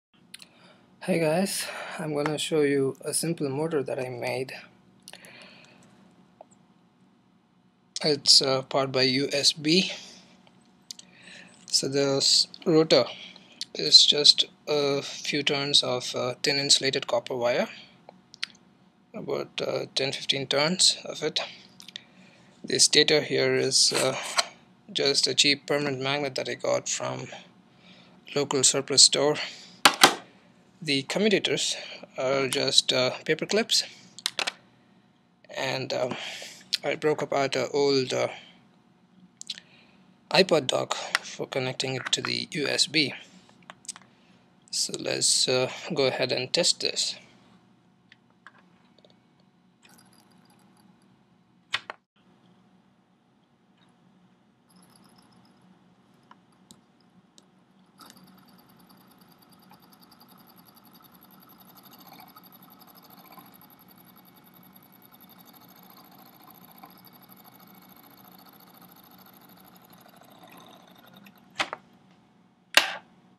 simple electric motor powered by usb
A simple electric motor made from paper clips, wire and magnets. Powered by USB.

Caution: Don't extract too much power and blow away your USB port !